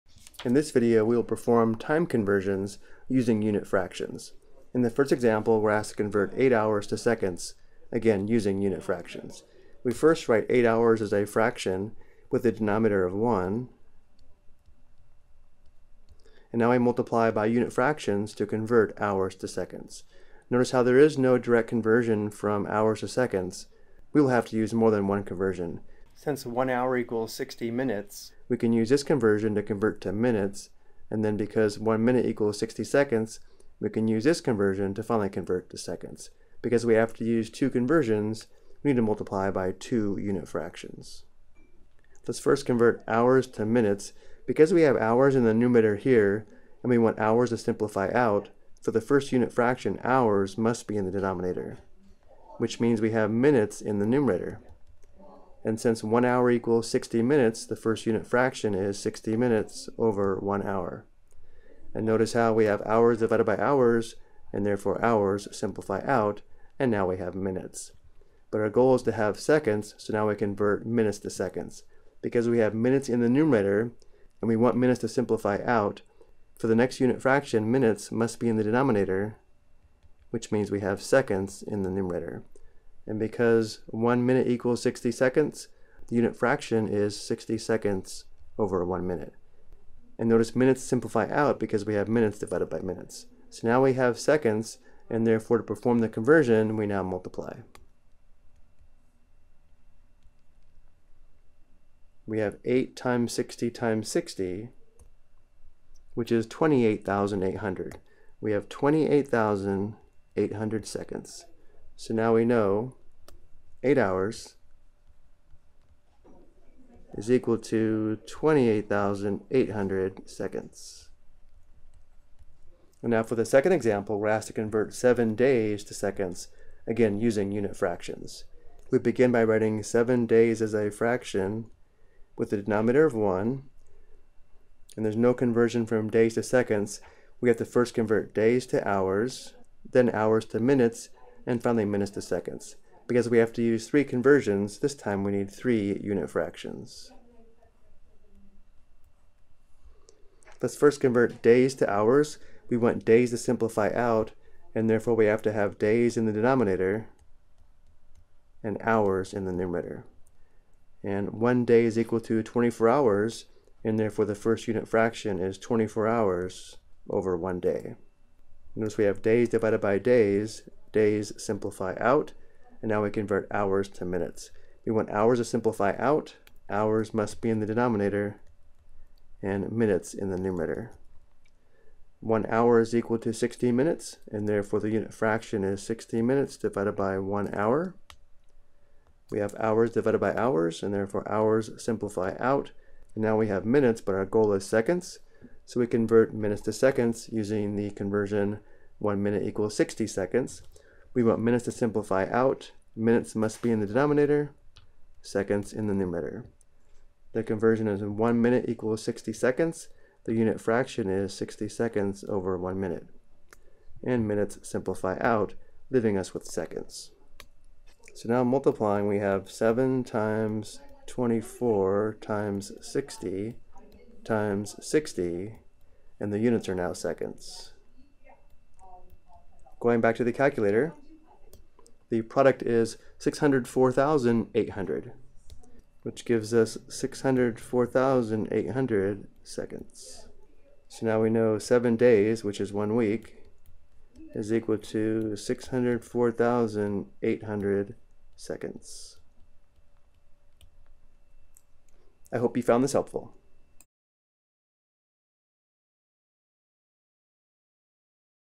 Convert Hours and Days to Seconds using Unit Fractions
This video explains how to use unit fraction to convert units of time.